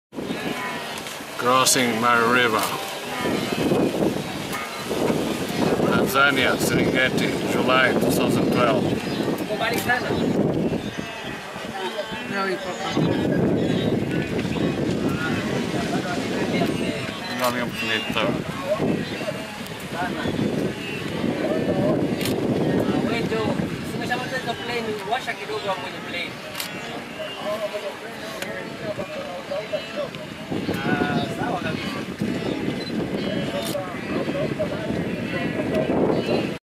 SERENGETI - Gnu Crossing 5/5 - 1ST August 2012
5 th Morning Gnu/Wildbeast Crossing of the Mara River of the 1st of August 2012 from Serengeti to Masai Mara - Serengeti National Park - Tanzania - Africa